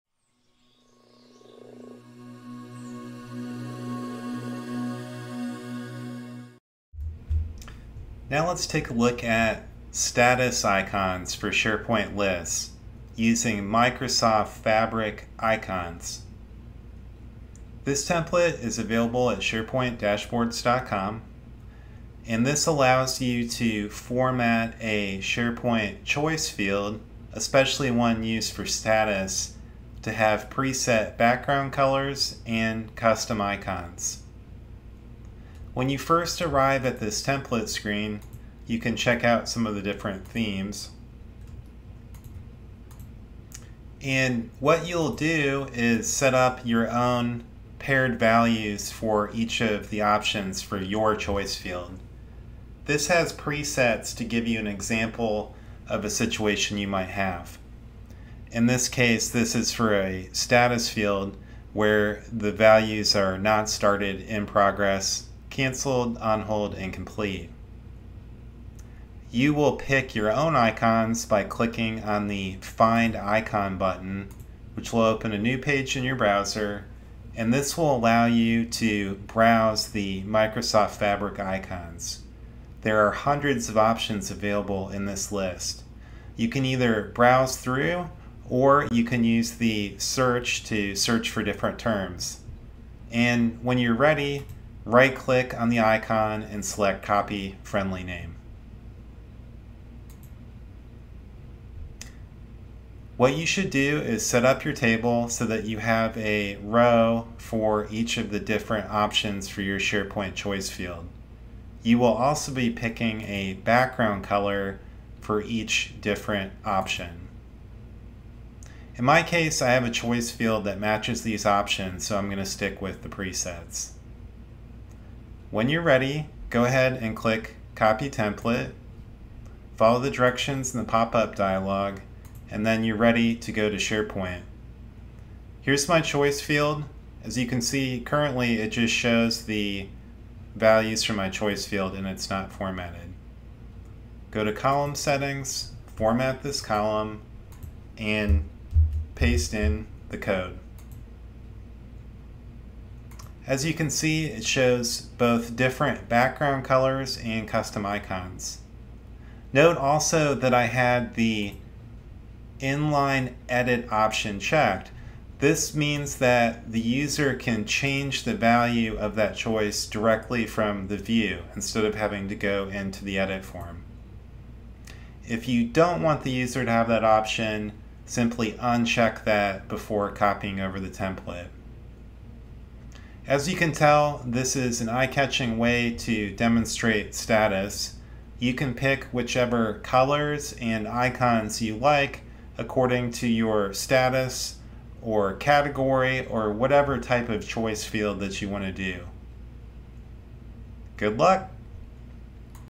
Status Icons using Fabric Icons in SharePoint Online Microsoft Lists Modern List View
Learn how to add a simple Status Icons using Microsoft Fabric Icons in your modern SharePoint list view. This template is available here: